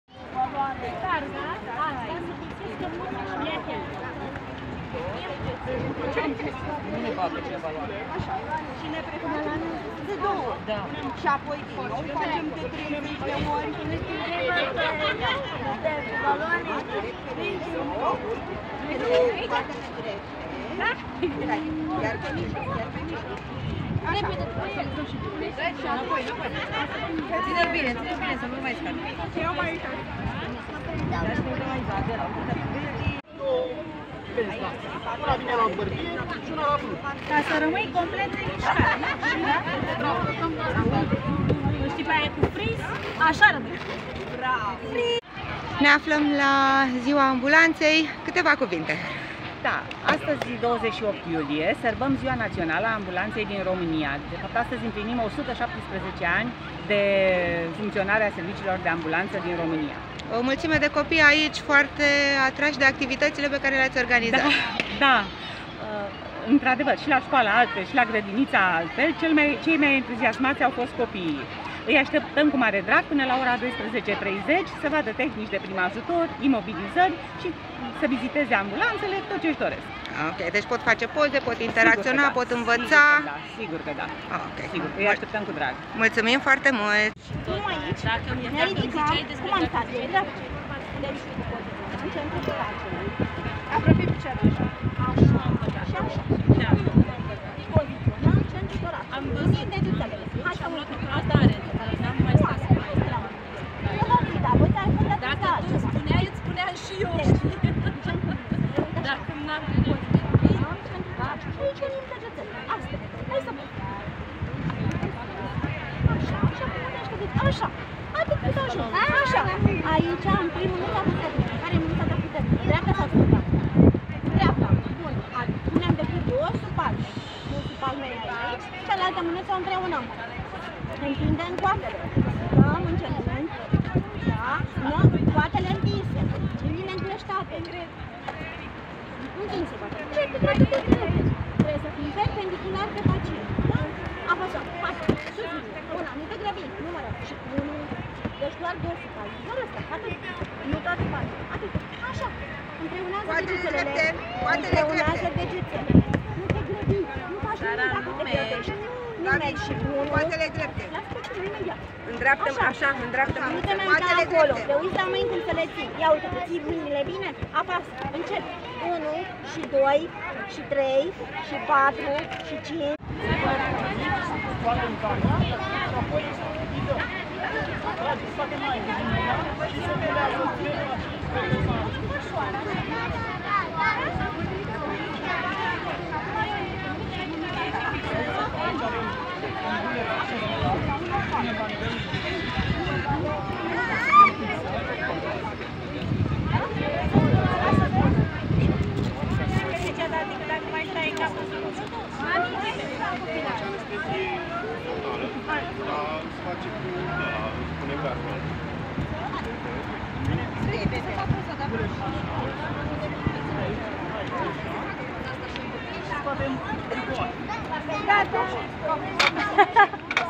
Ziua Națională a Ambulanței
Ziua Națională a Ambulanței a fost celebrată în Ploiești vineri, 28 iulie, pe esplanada Palatului Culturii. Zeci de copii au participat, entuziasmați și mai ales curioși să învețe tehnici de prim ajutor, să vadă echipamentele medicale folosite de echipele Ambulanței Prahova, dar și să intre în interiorul autospecialelor pentru a vedea cum este într-o salvare. Doritorii, adulți și copii, au primit chiar și investigații gratuite: măsurarea tensiunii, a pulsului și a oxigenului, iar pentru ca experiența să fie cât mai plăcută, au primit dulciuri.